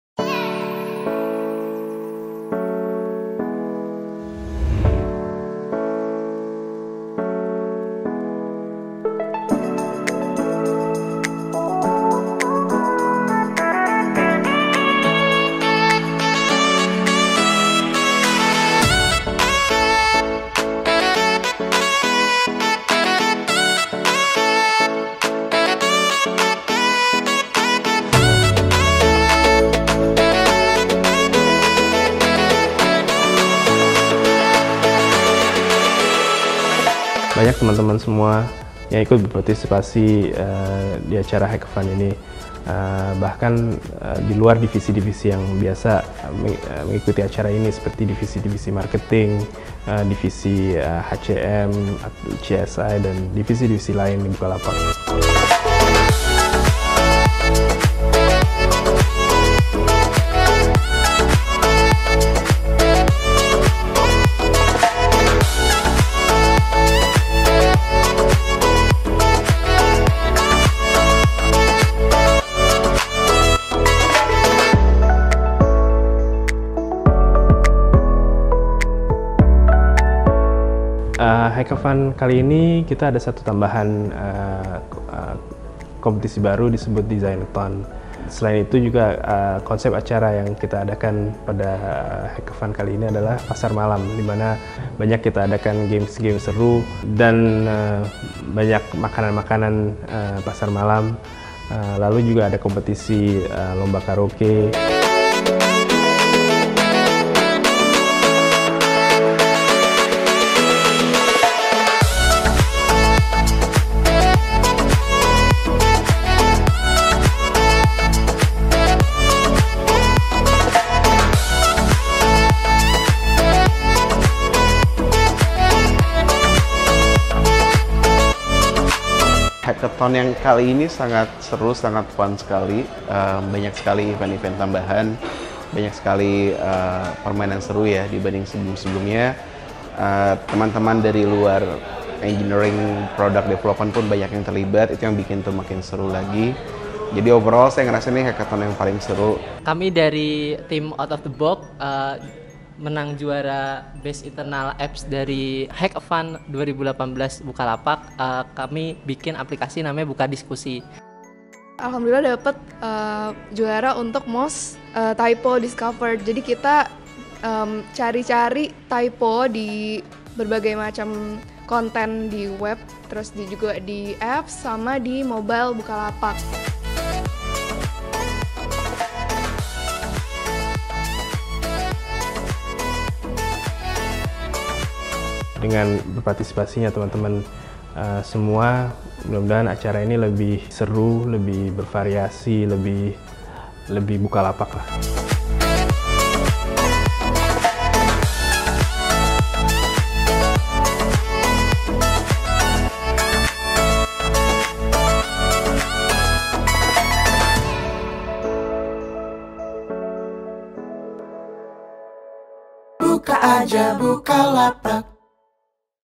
Hack-A-Fun Bukalapak 2018
Hack-A-Fun adalah event tahunan yang diikuti oleh karyawan Bukalapak yang berisi Hackathon, Testathon, Designathon dan lainnya. Hack-A-Fun tahun ini bertemakan Pasar Malam dengan kemeriahan hiburannya, seperti fun games, lomba karaoke, streetfood corner dan aktivitas lainnya.

Hack-A-Fun tahun ini jadi makin seru karena pesertanya tak hanya dari Divisi Engineer & Product saja, tapi dari divisi lain seperti Marketing, HCM, CSM, dan lainnya juga turut berkompetisi menjadi jawara di Hack-A-Fun tahun ini.

Event Hack-A-Fun dijadikan sebagai ajang berkompetisi dalam mengimplementasikan ide kreatif untuk Bukalapak! Inilah para juara Hack-A-Fun Bukalapak 2018:

**HACKATHON:**

- Juara I Best Internal Bukalapak App: [Out of The Box] Diky Arga Anggara, Reza Faiz Atta Rahman, Verra Mukty, Christoper Hans, Rasyid Ridho
- Juara II Best Internal Bukalapak App: [Winnie The Pooh] Mochamad Try, Andree Surya, Irsal Shabirin, Zulva Fachrina, Raihannur Reztaputra
- Juara II Best Internal Bukalapak App: [DemiHCM] Belinda Rahmadara, Ricky Arifandi Daniel

- Juara I Best Interaction & Design: [Hayuperang] M Zahid Rausyanfikri, Nathanael Krisna, Iswahyudi, Syafrizal Wardhana, Muh Mirza, Auditya Dino, Riyani Mardikaningrum
- Juara II Best Interaction & Design: [Apalah Arti Sebuah Nama] Arga Asasta, Mega Annisa, Amirul Muhammad, Anno Musa, Surya Thiono Wijaya, Michael Ryan

- Juara I Best GMV Growth Hack: [Tahu goreng pak jimb] Aldy Syahdeini
- Juara II Best GMV Growth Hack: [King Joffrey] Brurce Abraham, Putu Harum Bawa, Stepanus Jeffry, Adhika Aryantio , Destya Danang Praditya

- Juara I Best Technical Debt Killer: [Mobile First] M. Rizky Habibi
- Juara II Best Technical Debt Killer: [nguing-nguing] Hafidh Rashemi Rafsanjany, Arief Yudha Satria, Nur Siti Muninggar, Putri Wikie

- Juara I Best Digital Service: [Power Ranger] Marchellin Antonia, Sumanto, Selamet Riyadi, Regina Avena
- Juara II Best Digital Service: [Gedebug] Muhammad Rasyid Ridho, Krisbianto Cahyo Sumarto, Sofia Rhamadani, Rizky Kharisma, Maulana Solihin H
- Juara III Best Digital Service: [KPKB] Teguh Nugraha, Khairul Annas, David Christopher, Octavianus Putera Wardanna
- Juara III Best Digital Service: [LAB] Boby Indra Pratama, Astandro Koesriputranto, Luthfan Aufar

**TESTATHON:**
- Juara Most Innovative: [FunTesting Four] Aditya Mirza Bahari, Abid Nurul Hakim, Revan Bagus Amrulloh, Taufiq Elrahman

- Juara I Total Bug Report Point: [Super Timy] Timothy Pratama Widjaja, Aditya Amin Refandi, Intan Amalia Sari, Ulva Helena Sianturi, Anang Ferdi Kusuma
- Juara II Total Bug Report Point: [Eh Ada Orang] Arkadius Reymond, Niken Paramita, Raymond Lucky Sumargo
- Juara III Total Bug Report Point: [FIX You] Fixco Candra, Candra Dwi Prasetiyo, Leli Maharani, Mukimah Rizky Takkini, Noviyani Witayati
- Juara Harapan I Total Bug Report Point: [RIFLE] Fathir Wafda, Yulinar Elfrida, Lamhot JM Siagian, Ria Ratna Sari, Irwin Harnia Sari
- Juara Harapan II Total Bug Report Point: [SpeedTest] Tri Anugrah Teguh Pambudi, Fajar Kurniawan, Agung Enrico, Muhamad Amirul, Surya Thiono

- Juara Best Quality Bug Report: [The Mahe 1.0] Abdul Manan Maksum, Damarananta, Diah Fauziah, Dian Septiana Nugraha, Ferawati Hartanti Pratiwi

- Juara Most Typo Discovered: [QUARTET] Nirmalasari, Azalia Izzah Laila Ramadhani, Gadis Kumalasari, Febrina Ramadhani Putri

- Most Effective Team: [Tai-Can] I Wayan Ratmandala Lukman, Imam Mucholisin, Dea Nidya, Genio Erwanda, Andika Rachman

**DESIGNATHON:**
- Juara I Best User Centered Redesign: [Teman Danang] Tommy Anugrah Dwi Putra & Cinintya Putri
- Juara II Best User Centered Redesign: [Kerabat Danang] Syafrizal Wardhana & Katarina Menik
- Juara III Best User Centered Redesign: [Sahabat Danang] Mahisa Dyan Diptya & Afif adi
- Juara III Best User Centered Redesign: [Padawan] Handoko & Afnizar

Kamu juga bisa ikutan di Hack-A-Fun Bukalapak selanjutnya dengan bergabung menjadi bagian dari team Bukalapak di sini